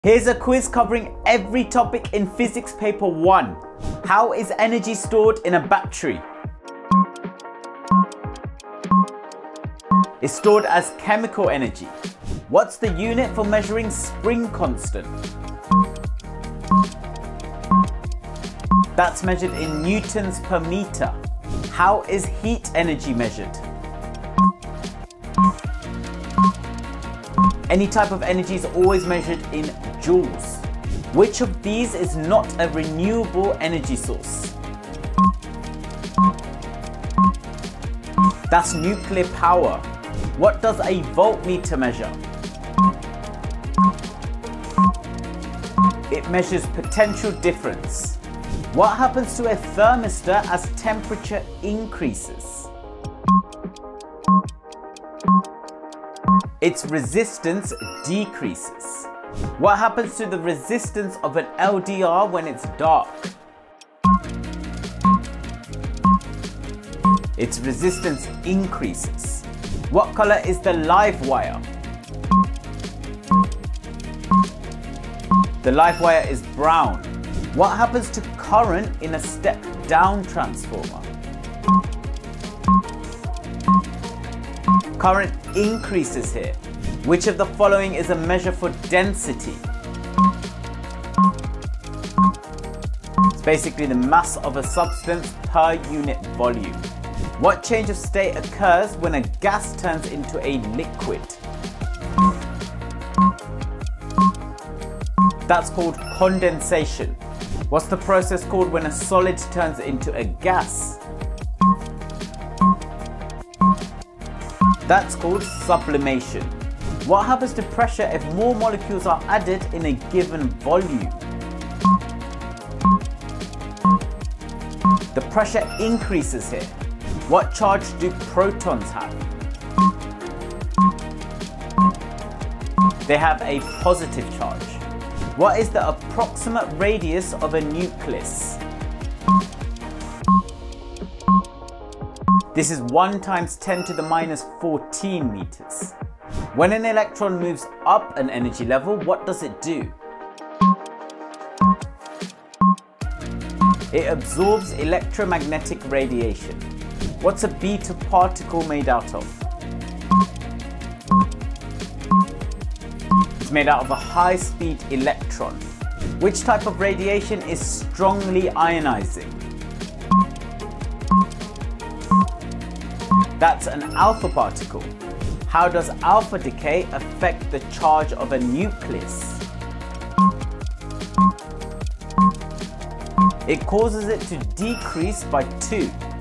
GCSE Physics: Exam Revision | Entire Physics Paper 1 Quiz
Test yourself using this quiz covering the Entire GCSE Physics Paper 1!